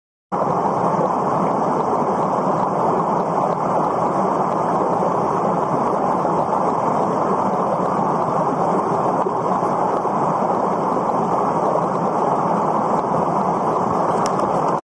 "Anolis Aquaticus" also known as the Scuba Diving Lizard can stay submerged underwater for around 15
"Anolis Aquaticus" also known as the Scuba Diving Lizard can stay submerged underwater for around 15 minutes. It breathes underwater through the pulsating air bubble seen on the top of its head.